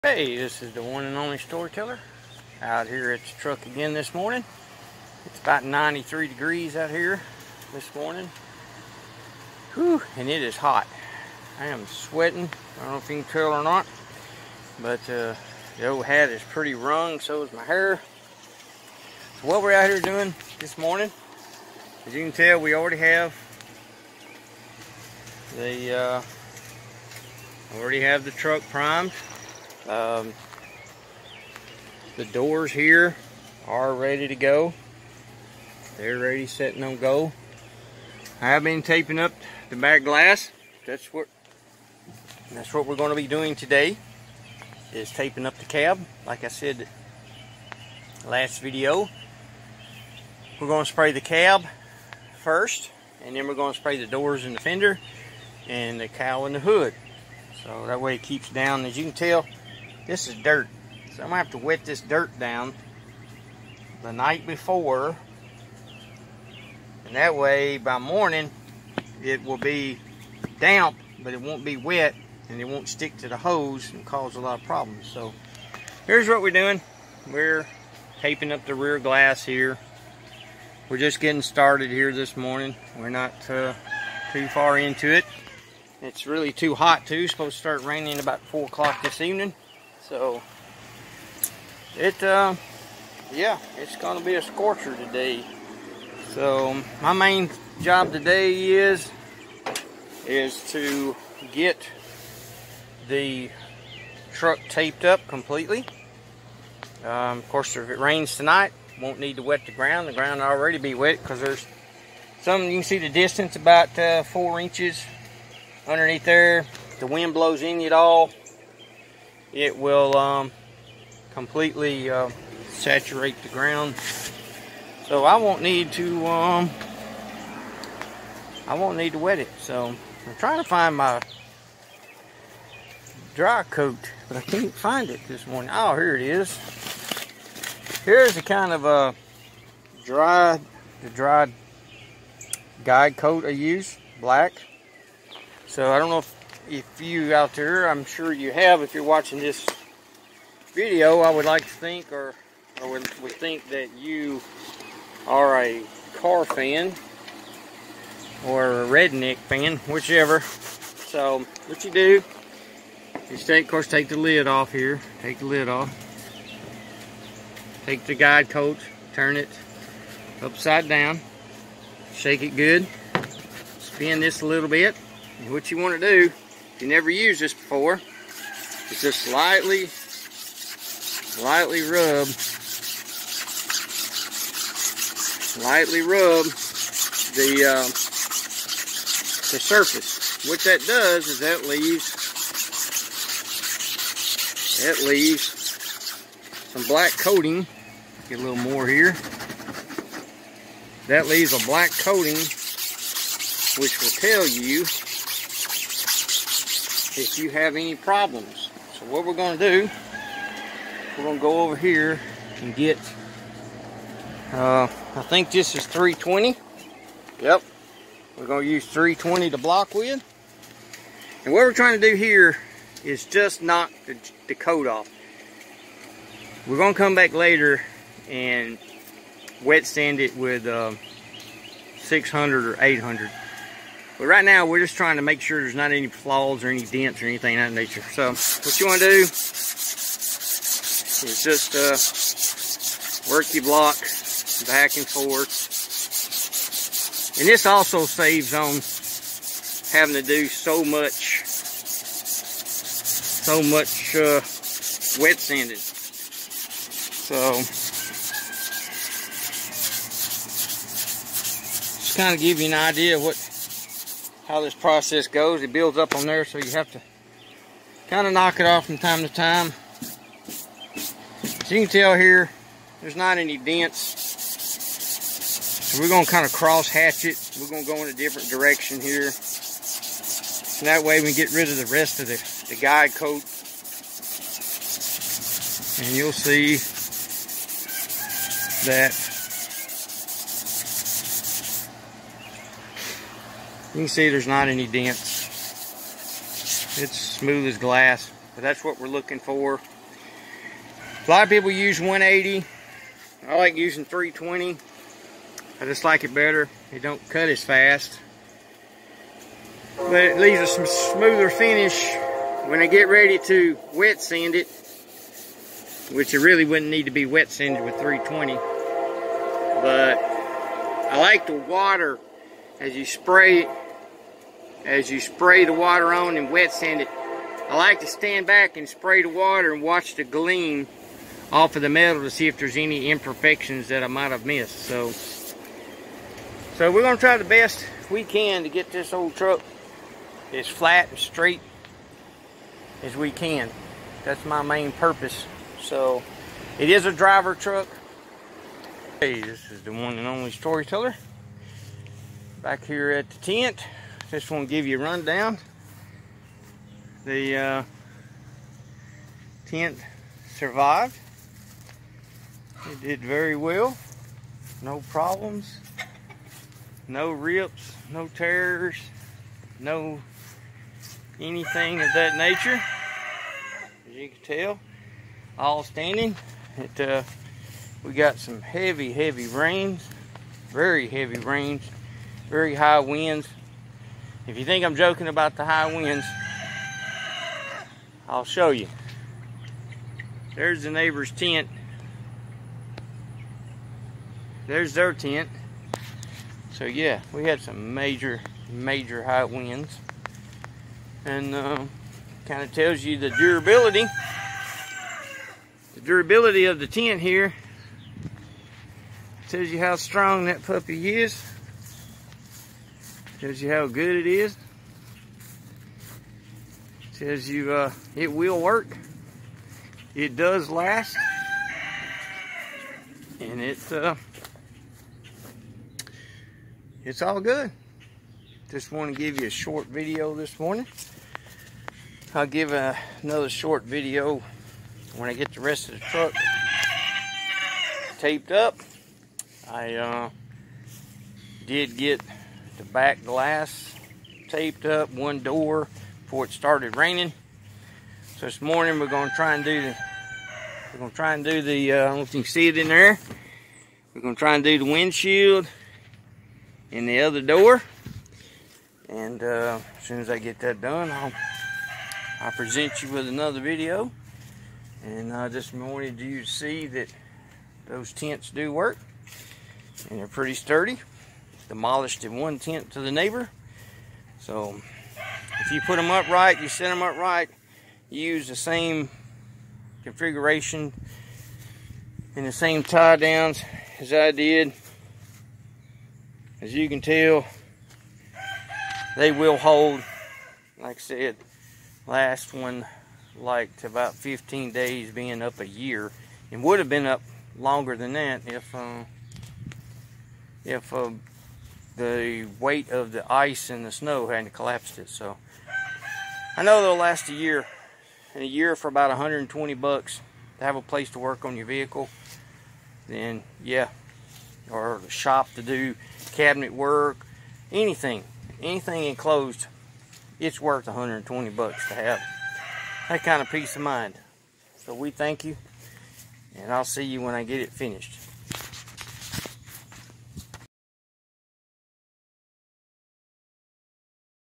Ford F150 1979 truck build part 33
Thanks for coming back, I hope you liked the Red neck tent series, we will be painting again on the truck I hope you like the video,